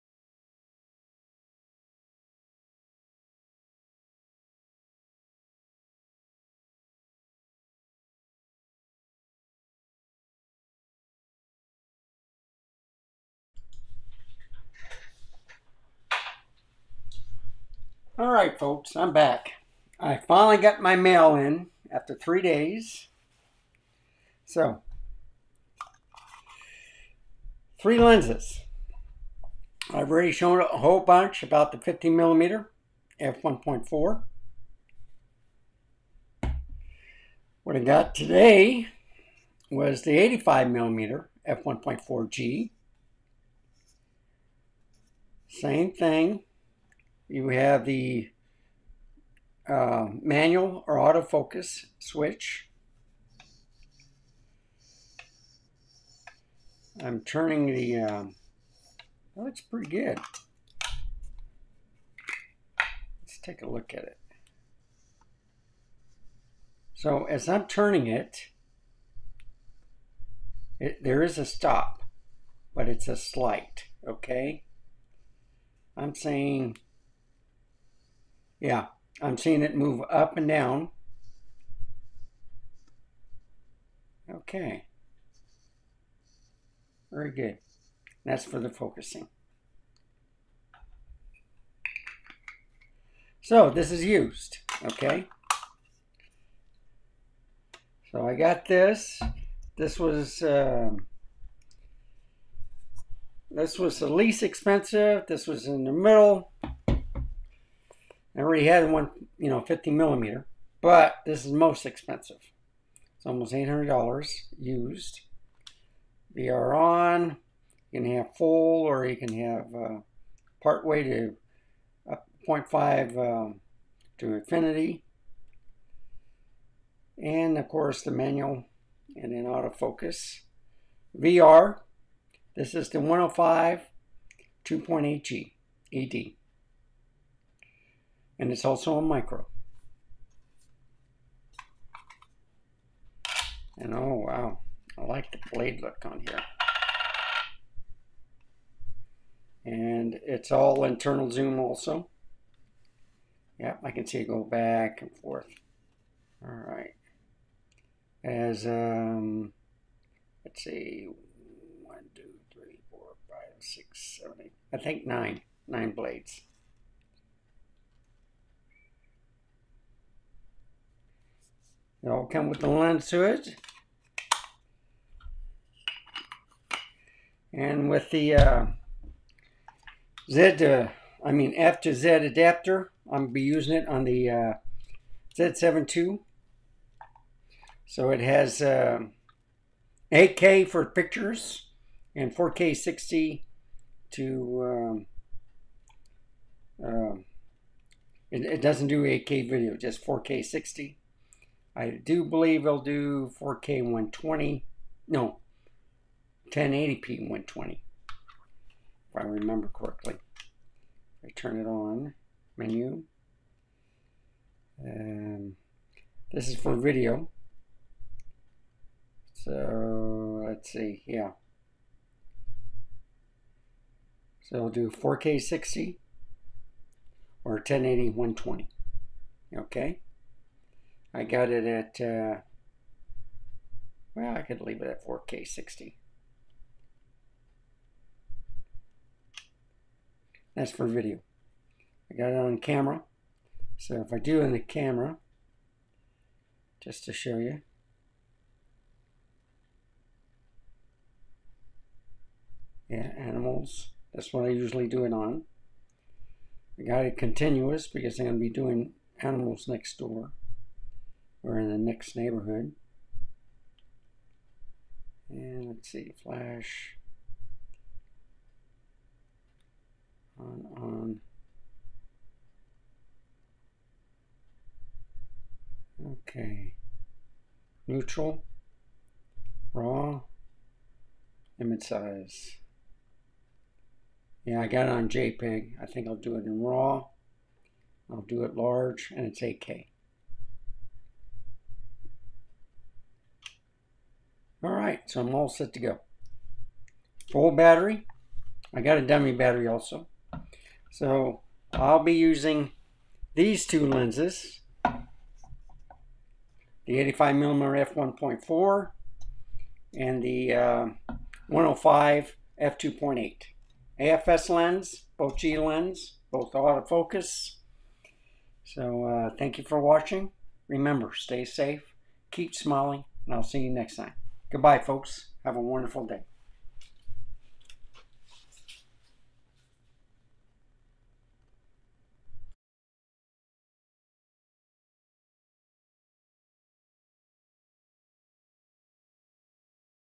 Mail Call Nikon AF-S Nikkor 85mm f/1.4G & Nikon AF-S Micro Nikkor 105mm f/2.8G ED VR on 2024/06/07
The Camera Exchange, Inc. local store mailed me these last two lenses to complete the F Mount Collection using adapters between multiple camera brands which two are its own, Nikon Z and Nikon 1.  Surprise!  Stay Safe and Keep Smiling!  Cheers!